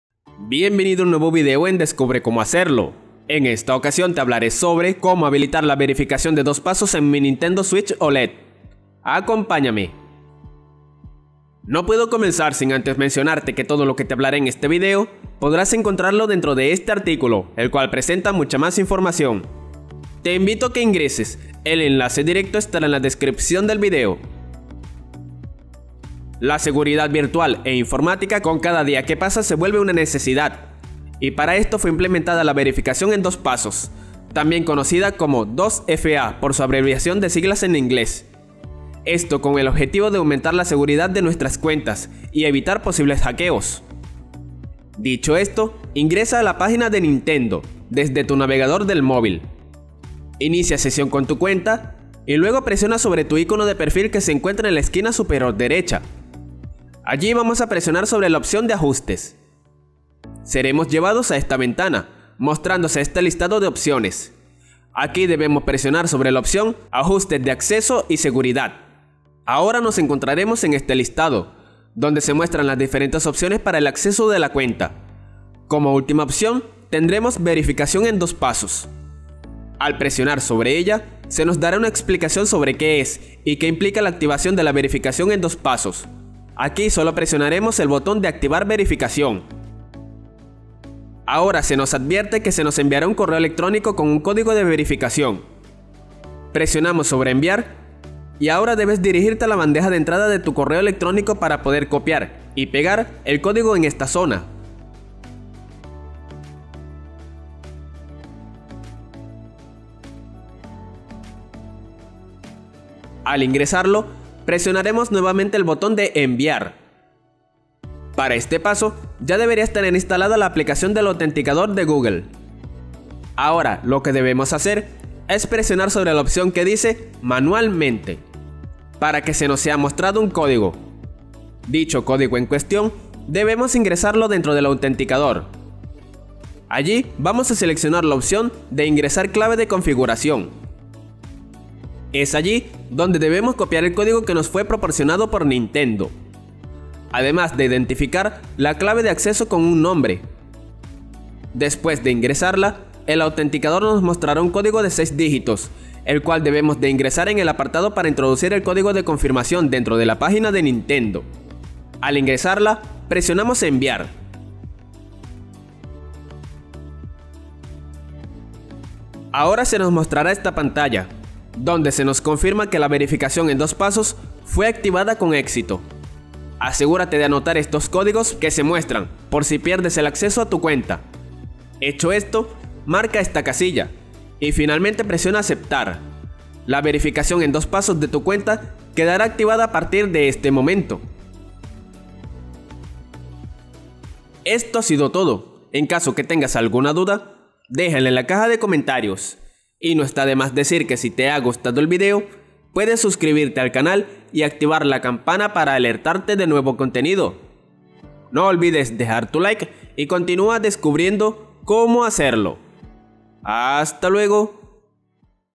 ¿Cómo habilitar la verificación de dos pasos en mi NINTENDO SWITCH OLED?
En este video descubre Cómo habilitar la verificación de dos pasos en tu NINTENDO SWITCH OLED. En el Canal Descubre Cómo Hacerlo encontrarás el mejor contenido. ¡No te lo puedes perder!